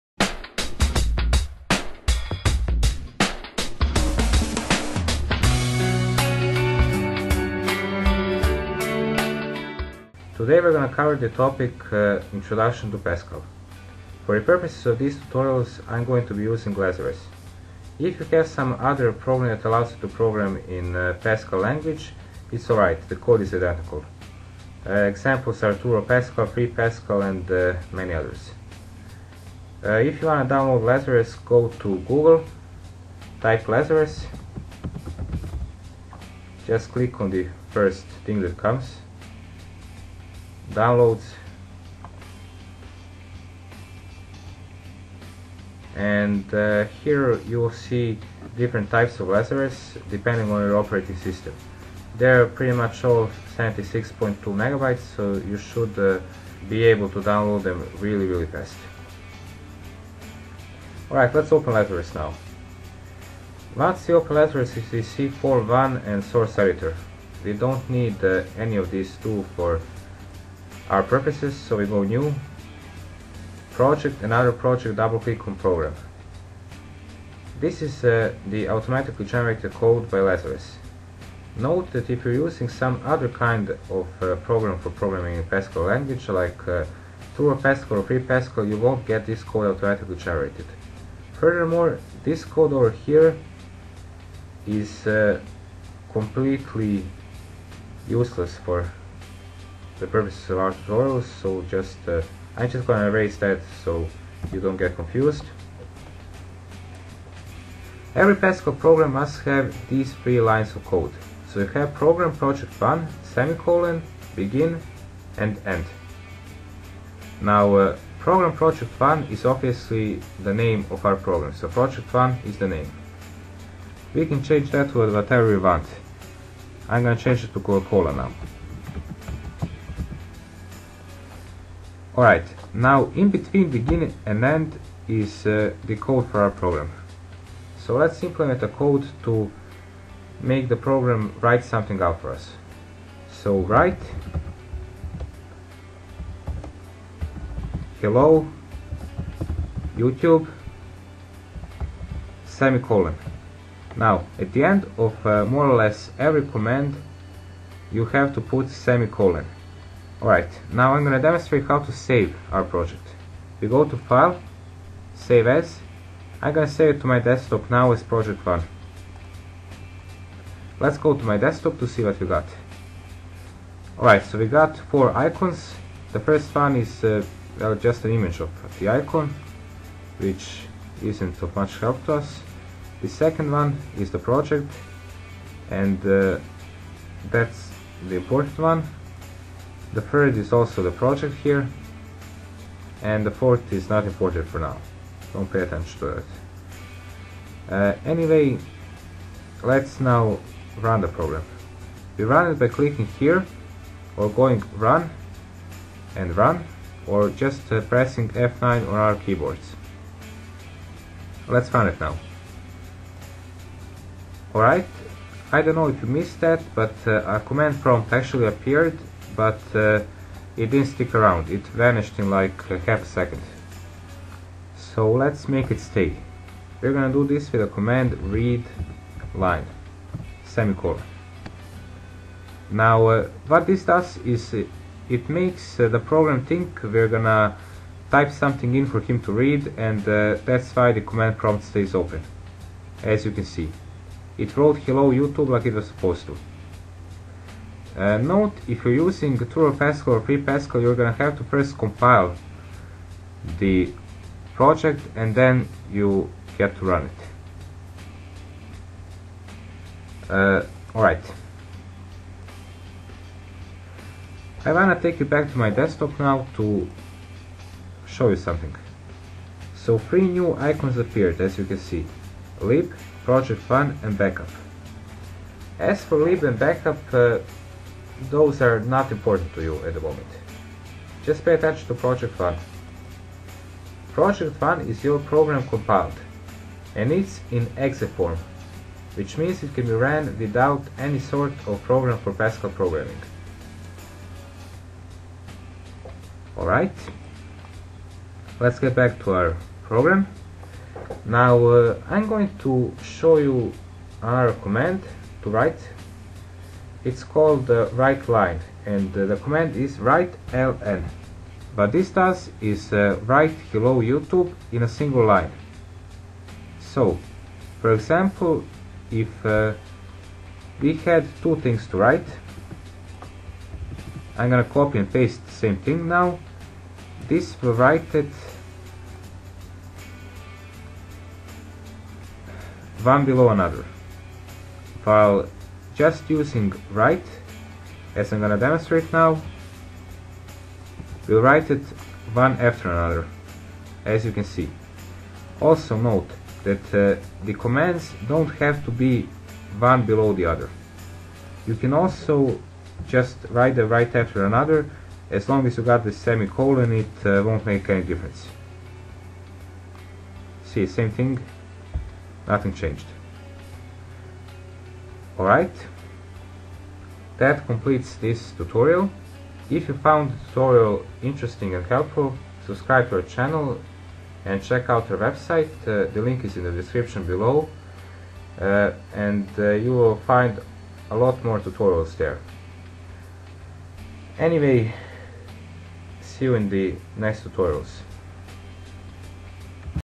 Introduction to Pascal - Tutorial - Pascal #1 - IT Lectures
This video will introduce you to pascal programming language and some of its  basic commands.